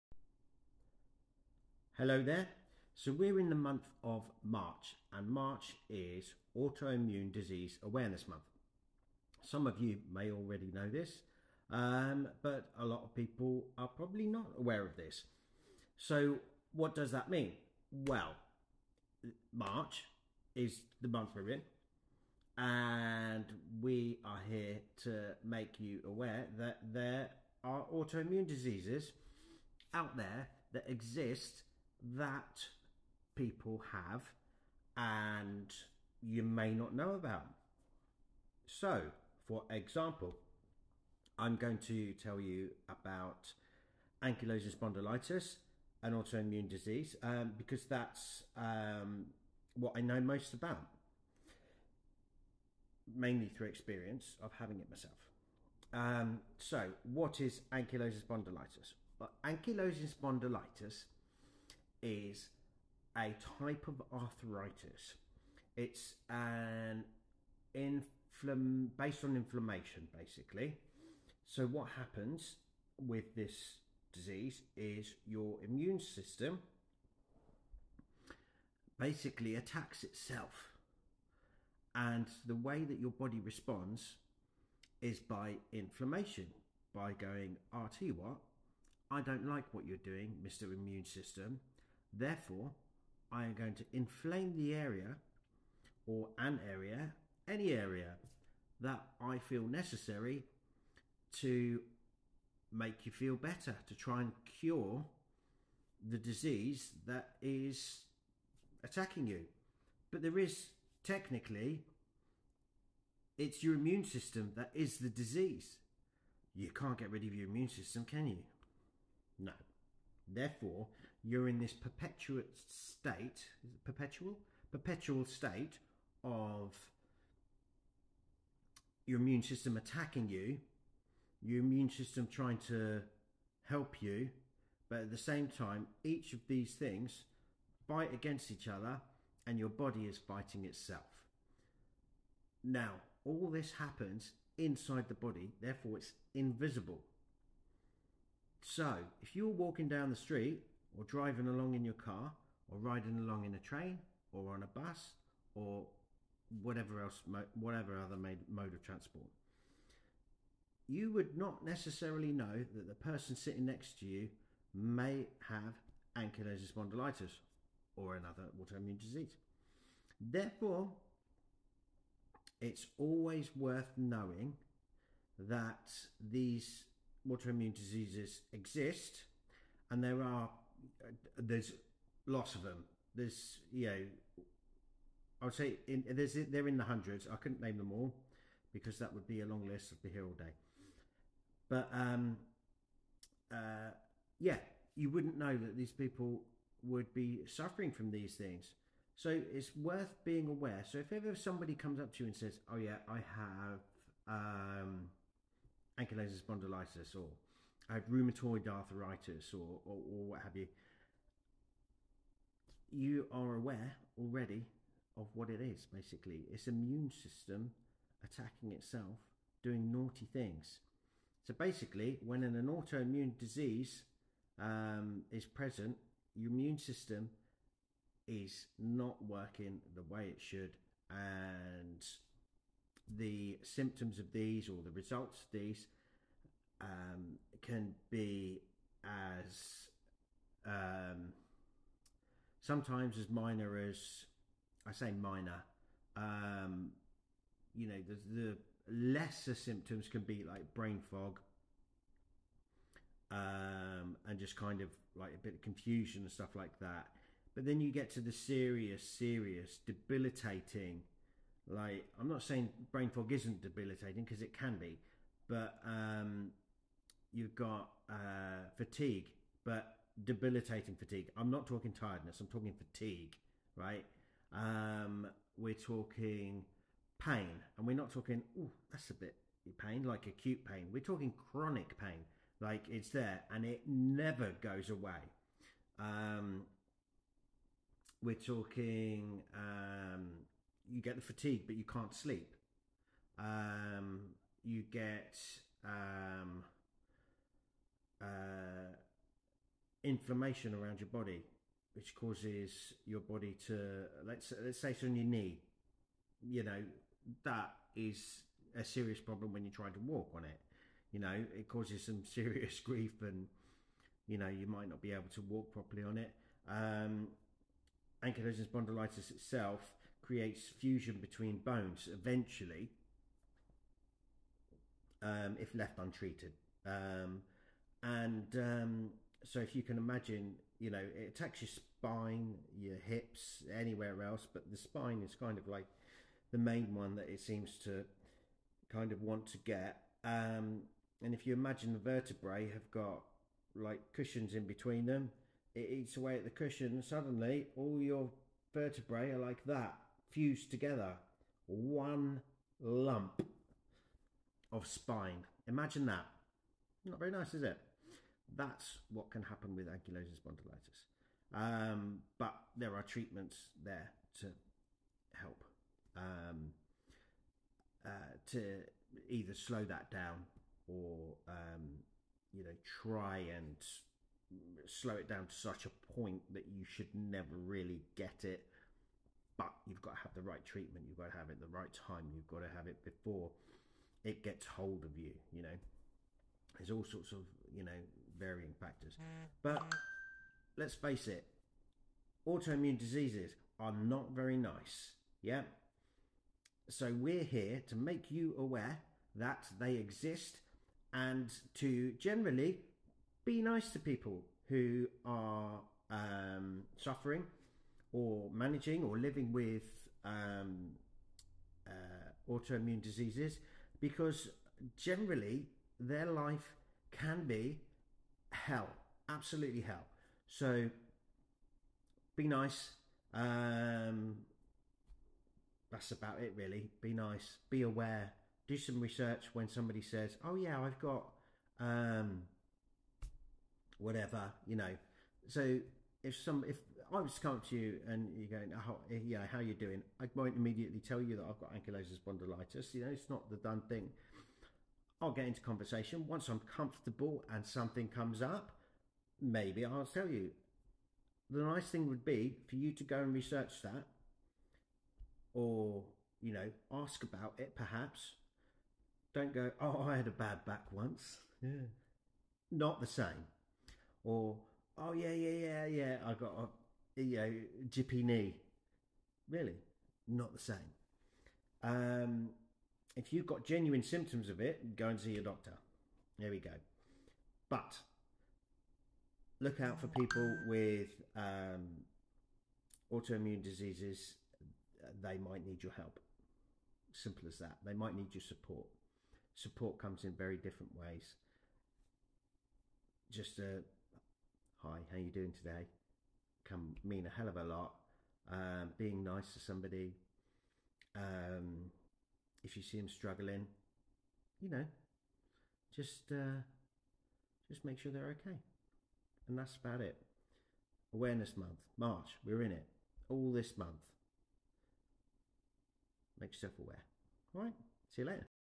March is autoimmune awareness month!
Just a quick reminder that this month is autoimmune awareness month.

do you know someone who has an autoimmune disease? are you aware of how it can affect them?